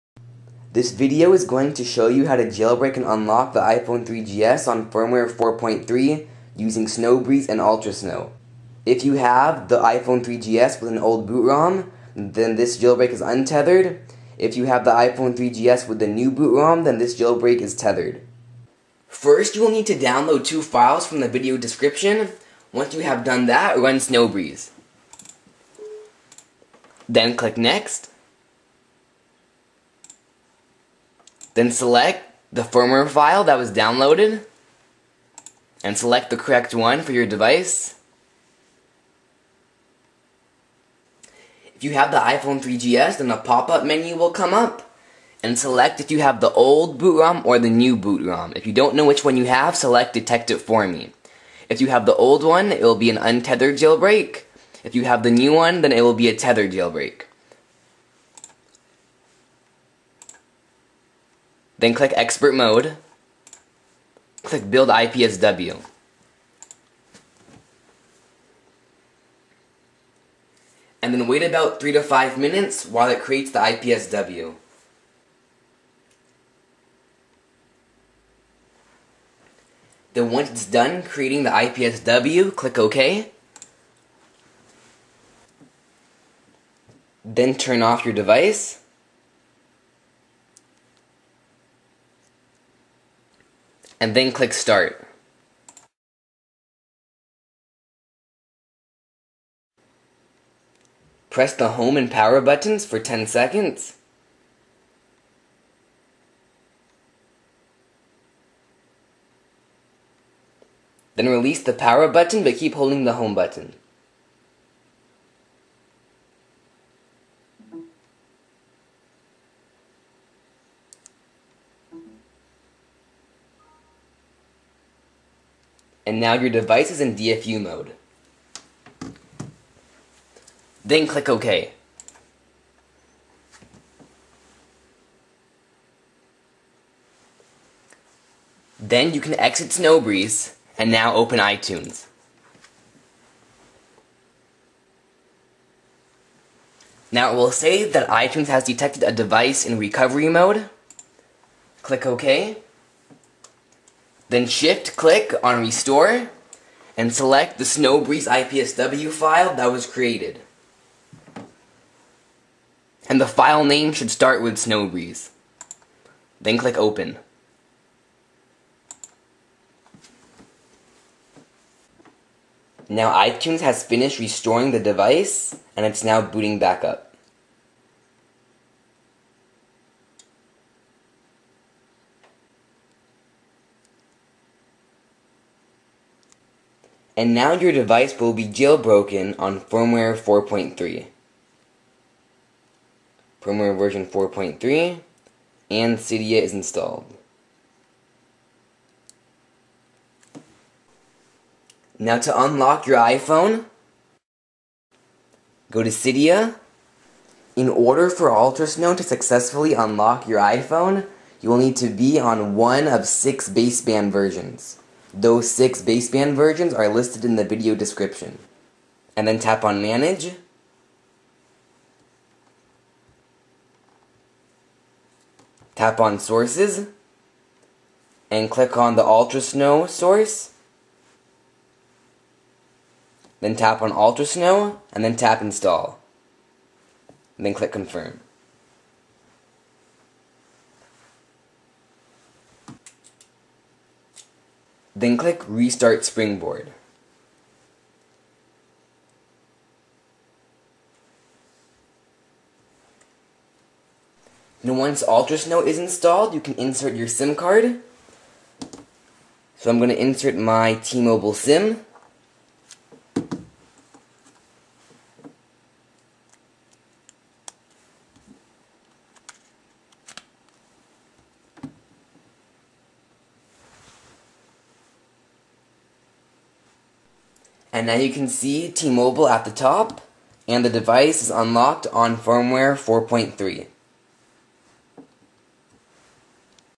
How To Jailbreak & Unlock 4.3 on iPhone 3GS (Untethered)
This video tutorial will show you how to jailbreak and unlock the iPhone 3GS on firmware 4.3 using Sn0wbreeze and Ultrasn0w. This jailbreak is untethered for the iPhone 3GS with an old bootrom. iPhone 3GS's with the new bootrom have a tethered jailbreak.

Add this source to Cydia and install Ultrasn0w (if it has not already been added): repo666.ultrasn0w.com

Ultrasn0w will unlock the iPhone 3GS on the following basebands:
01.59.00
04.26.08
05.11.07
5.12.01
5.13.04
06.15.00

Note: If  you rely on an unlock to use your iPhone, then you will need an official SIM card (such as an AT&T SIM) in order to activate the phone. Sn0wbreeze does not automatically activate the iPhone.

This video tutorial will show you how to jailbreak any device on firmware version 4.3 using Sn0wbreeze. This jailbreak will also enable multi-touch gestures on 4.3.

This jailbreak works on the following devices:
iPhone 4
iPhone 3GS (new and old bootrom)
iPad 1G

This jailbreak is an untethered jailbreak only for the iPhone 3GS with the old bootrom. This jailbreak is a tethered jailbreak for all other devices.

Note: If you have iPhone and you rely on an unlock, then you will need an official SIM card to activate the phone.

If you have the iPhone 3GS with a new bootrom (tethered jailbreak), then you will need to boot your device with iBooty after jailbreaking. Watch this video for instructions on how to boot up your device